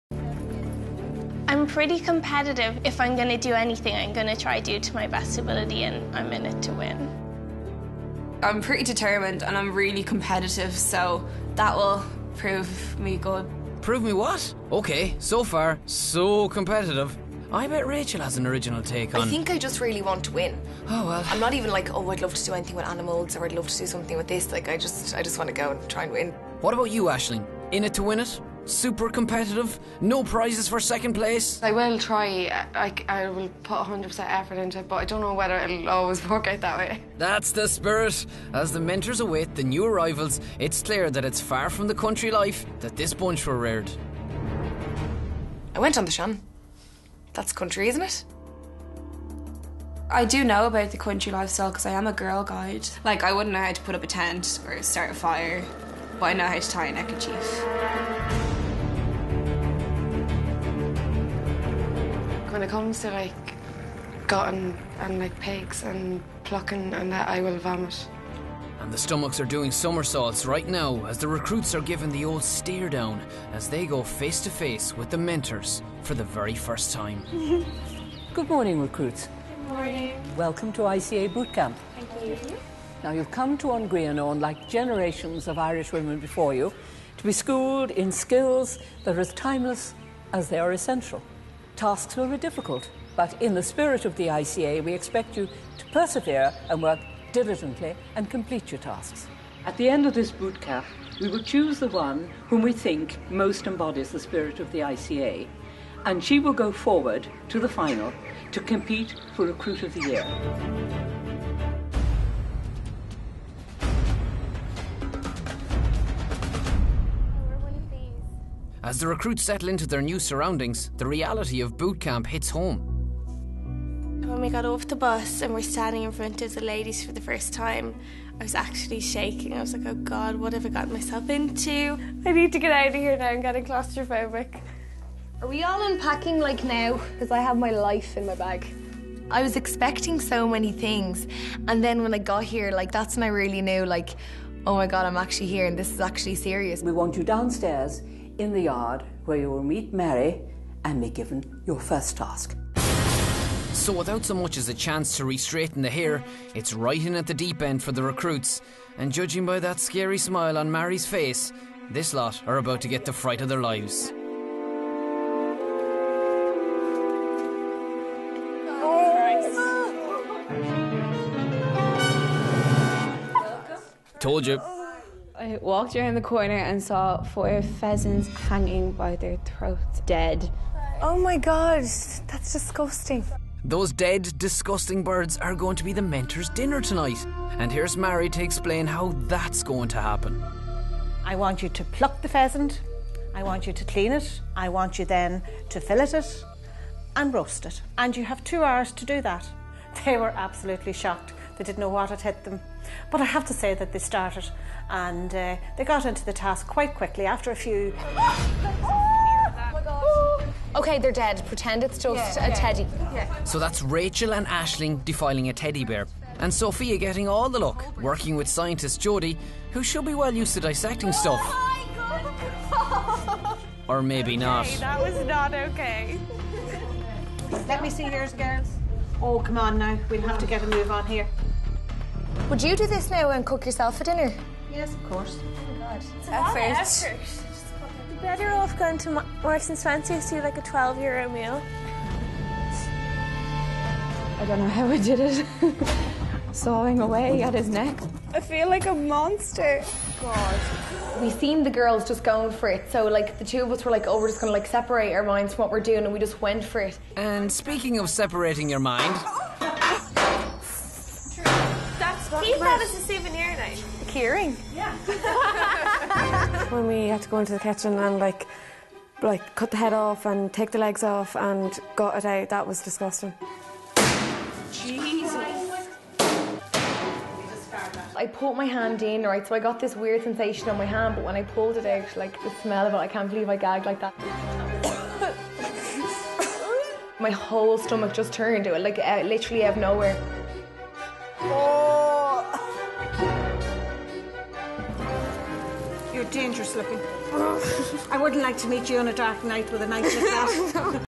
ICA Bootcamp series 2 episode 1
Aishling Jodie Rachel and Sophie tackle the pheasant !